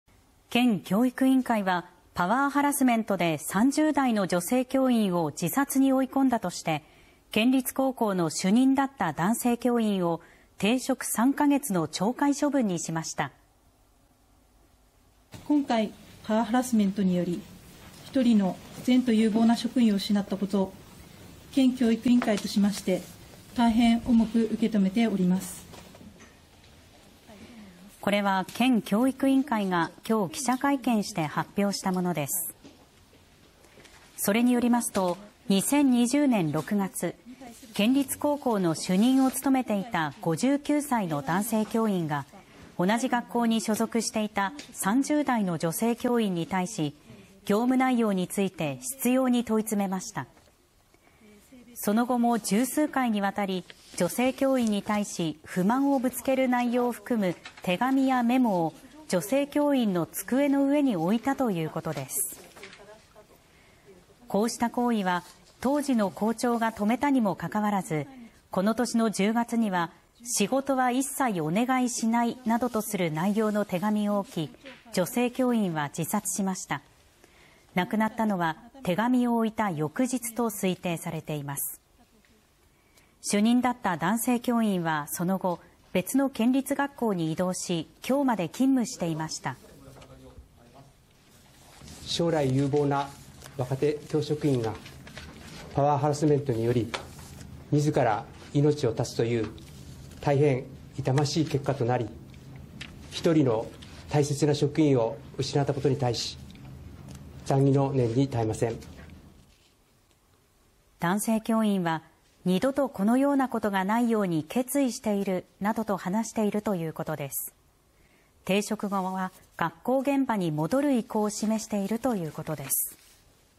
宮城県の高校で男性教諭のパワハラにより３０代女性教諭が自殺 男性教諭は停職３カ月
宮城県教育委員会はパワーハラスメントで３０代女性教諭を自殺に追い込んだとして、県立高校の主任だった男性教諭を停職３カ月の懲戒処分にしました。